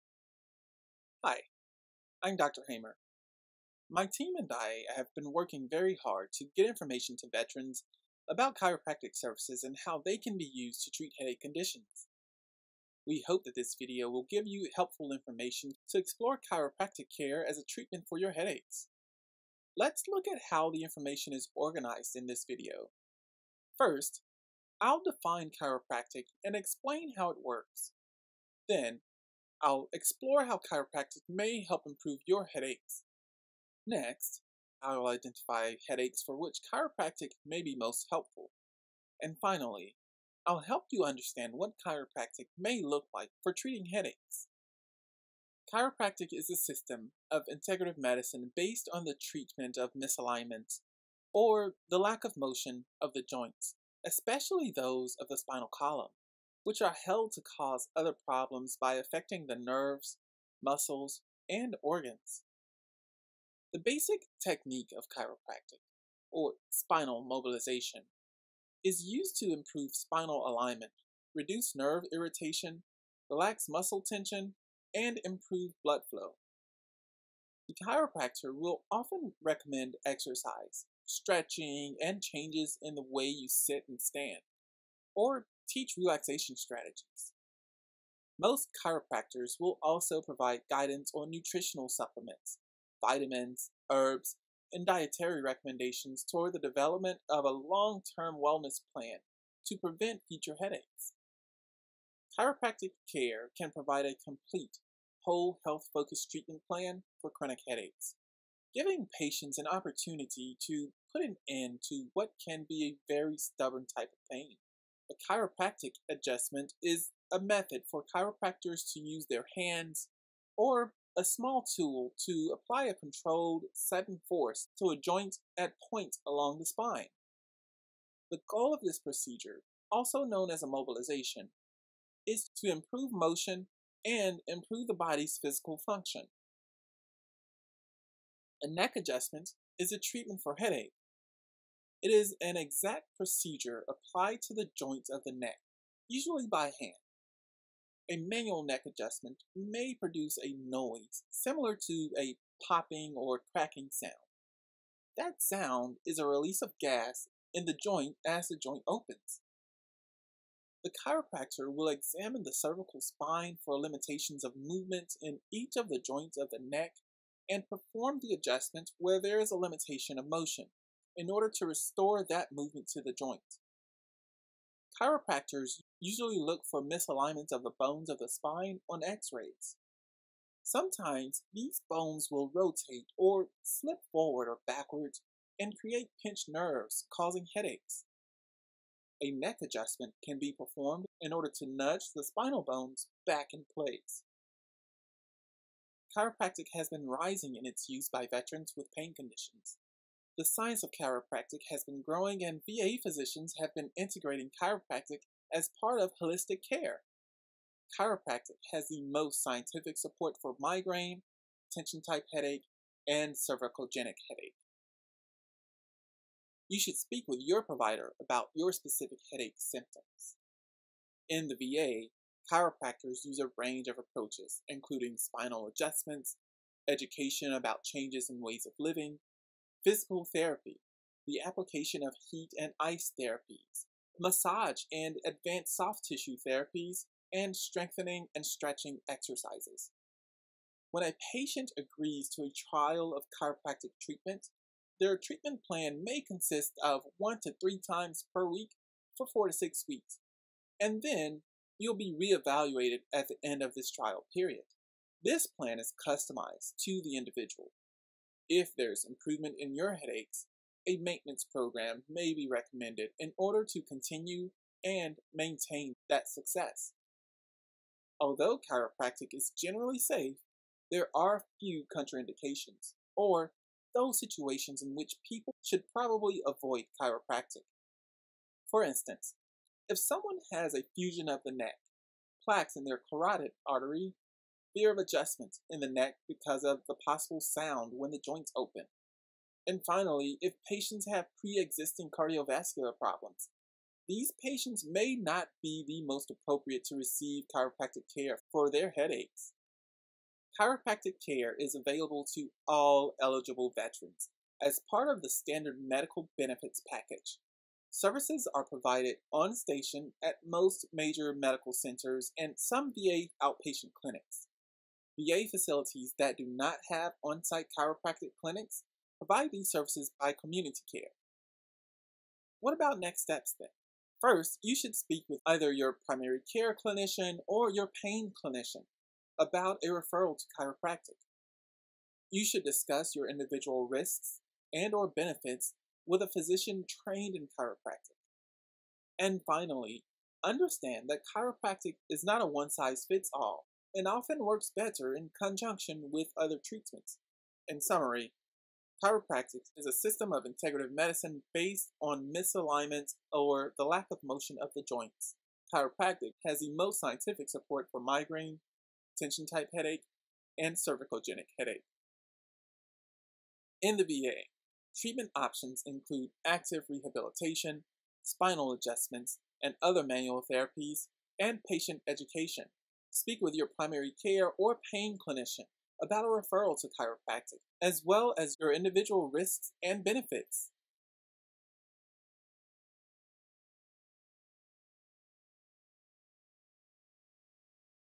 Chiropractic for Veterans with headache
This video will provide information to Veterans and healthcare clinicians about ways in which chiropractic may be used to decrease headache symptoms. This video was developed by Joshua Hamer, PhD, Archana Purushotham, MD, PhD, and Paul Sloan, PhD. This video was created with support from the VISN 16 South Central MIRECC.